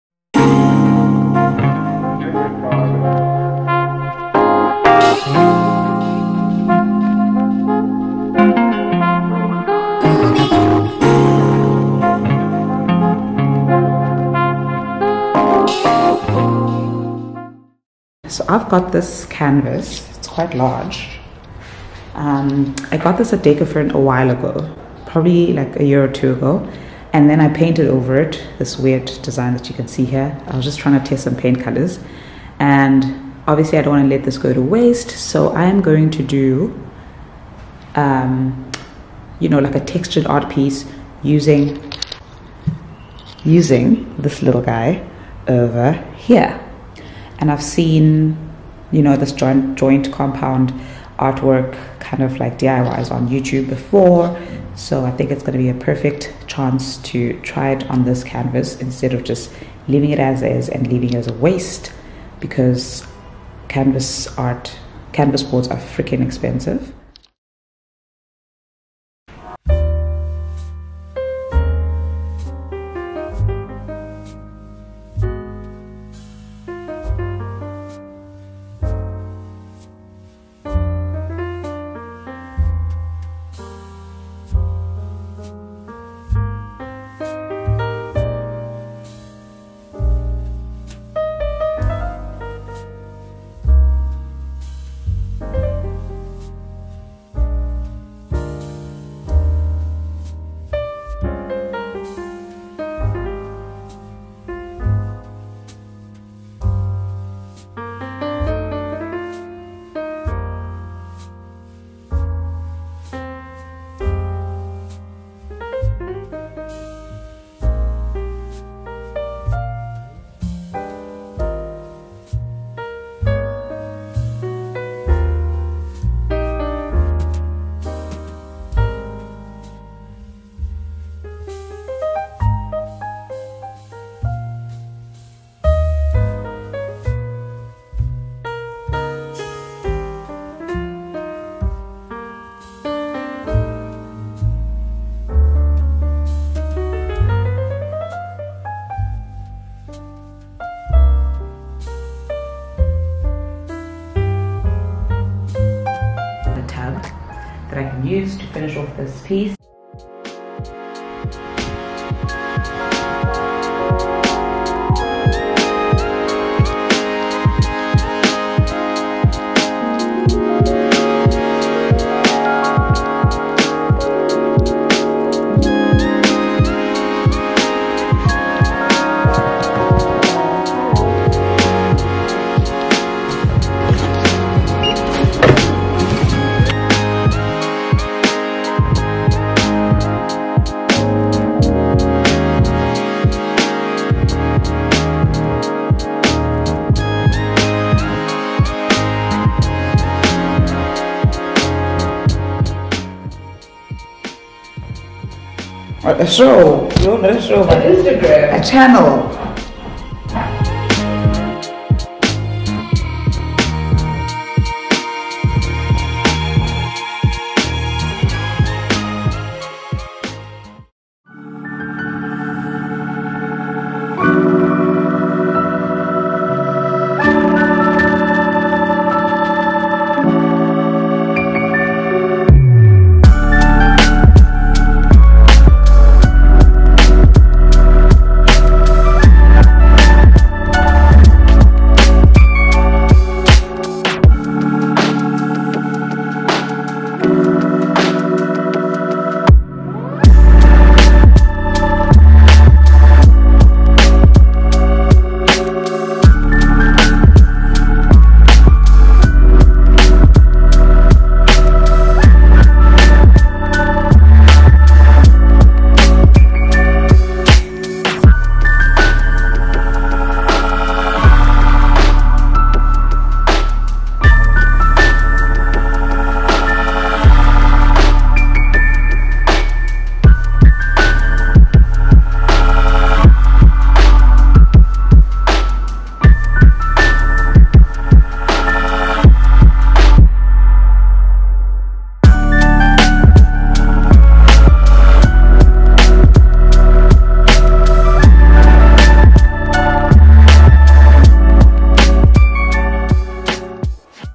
Home Decor Ideas | DIY Large Textured Wall Art| South African YouTuber
I've wanted to try this textured art  piece for the longest time and I'm so glad I finally did. I still need to frame and hang it but couldn't wait to show you guys. Let me know your thoughts and look out for the next video when I frame and hang it.

ITEMS USED:
Canvas board
Joint compound
White paint

FB: DIY Decor With Lorna

Personal page:
LornsKalimashe